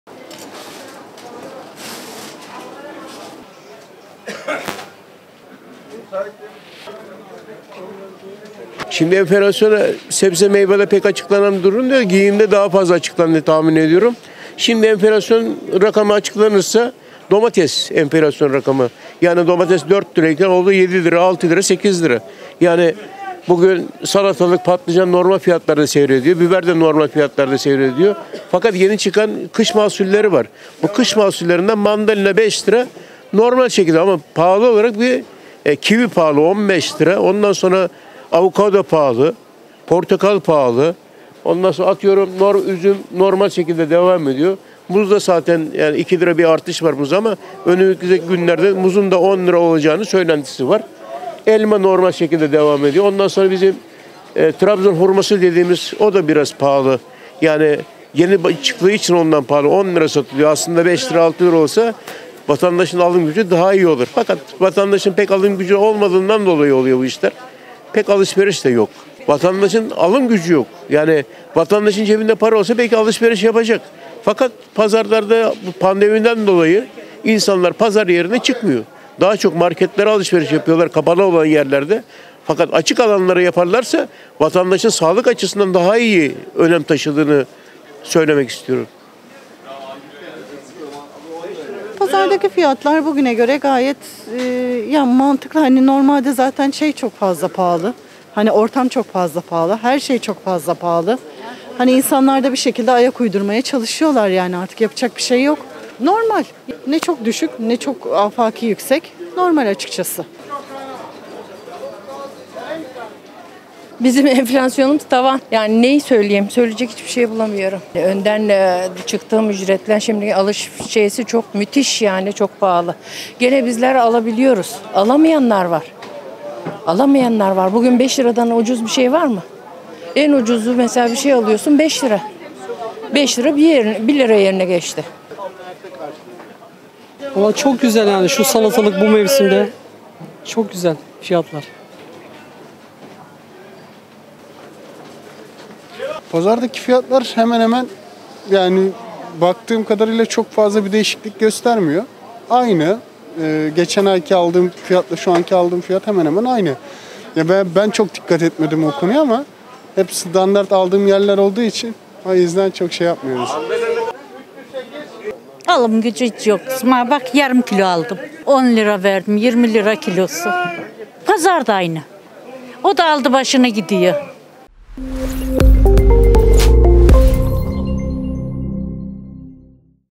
Bursalıların pazar enflasyonu
TÜİK, Ekim ayı enflasyon rakamlarını açıkladı. Buna göre tüketici enflasyonu ekim ayında yüzde 2.13, yıllık enflasyon ise yüzde 11.89 oldu. enBursa.com ekibi enflasyonun en çok hissedildiği pazarlarda Bursalılara mikrofon uzattı.